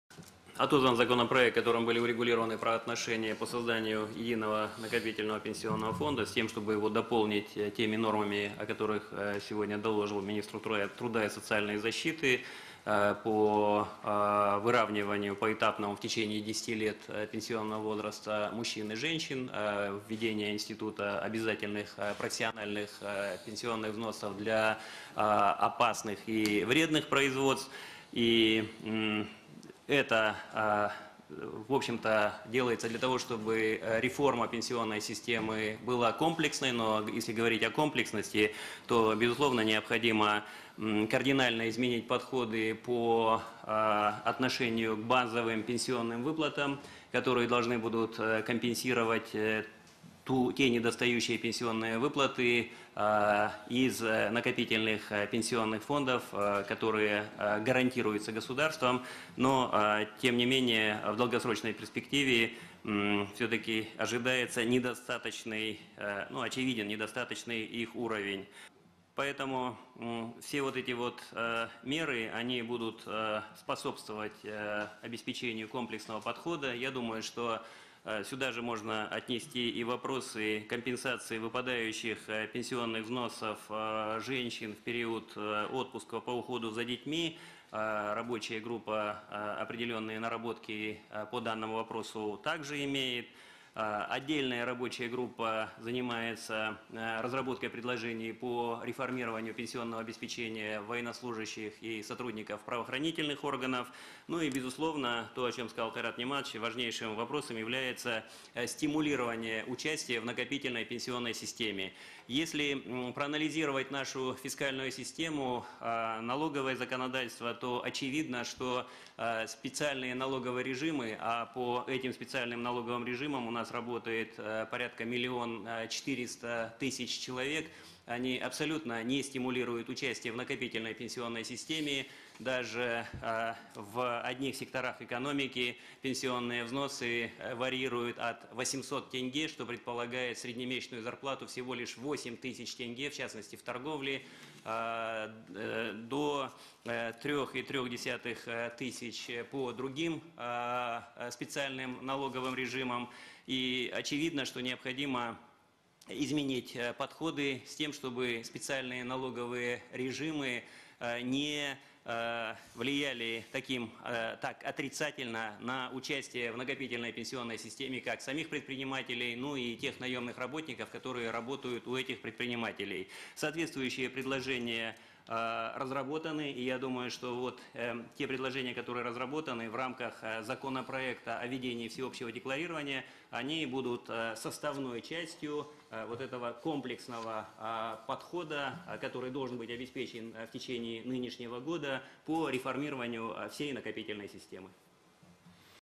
Выступление Б.Жамишева
На заседании Правительства министр финансов Болат Жамишев сообщил о проводимой работе по модернизации пенсионной системы.